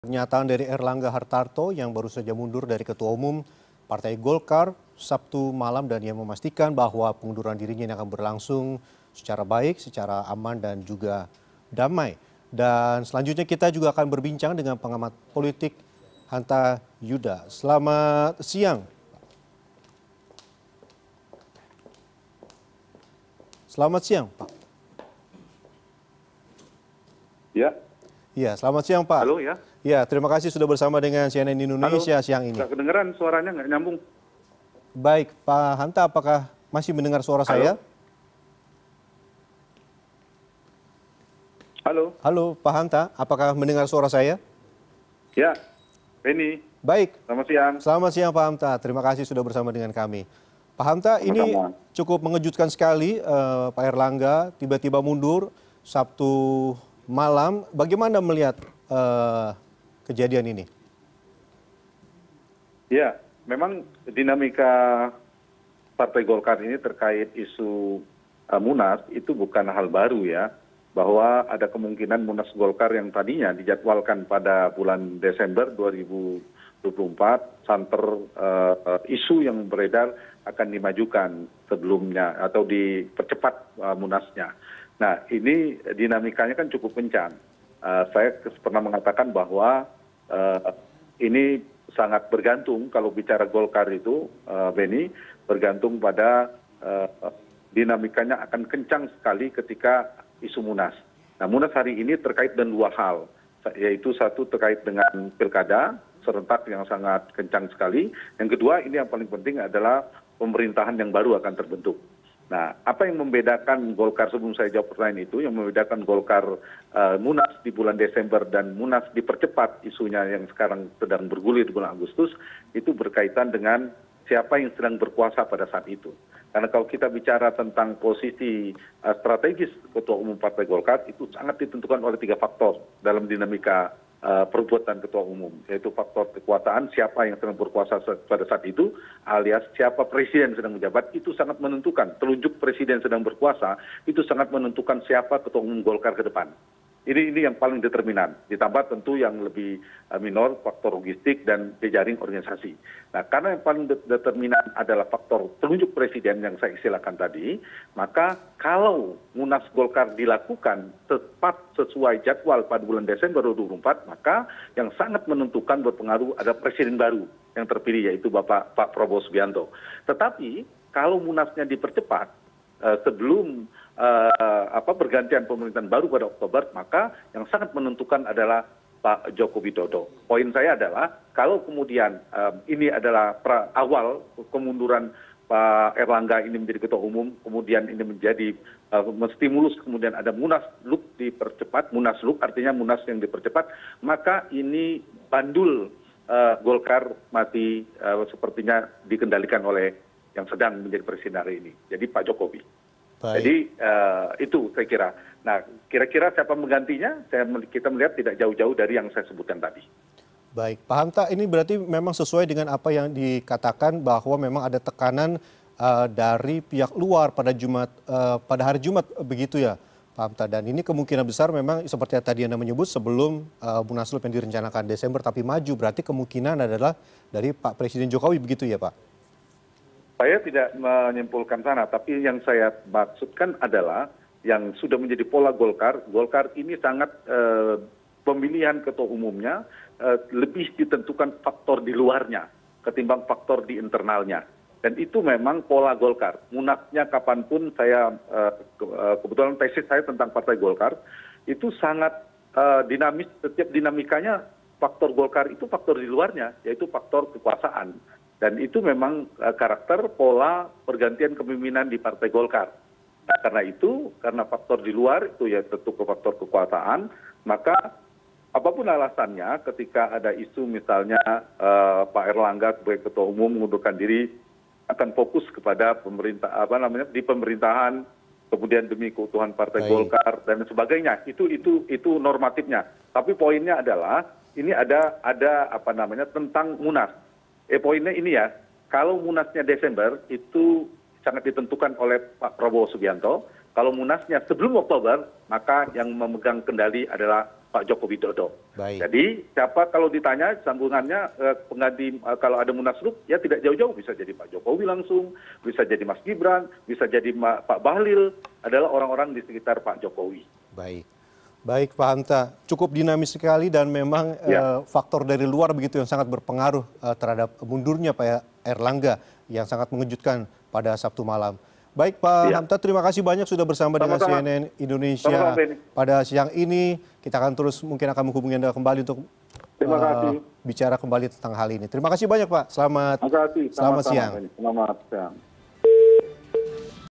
Poltracking Tanggapi Pengunduran Diri Airlangga Hartarto
Direktur Eksekutif Poltracking Hanta Yuda menanggapi pengunduran diri AIrlangga Hartarto sebagai ketua umum Partai Golkar.